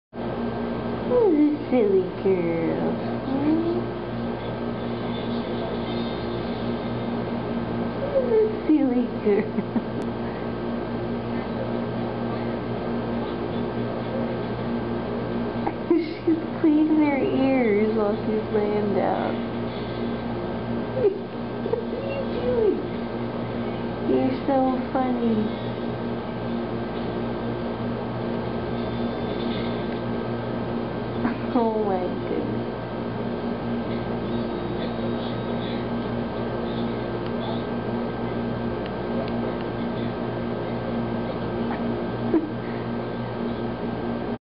Samantha grooms her ears while laying down!
Samantha is a bit lazy, and she often grooms her paws, face and even her ears while she's laying down!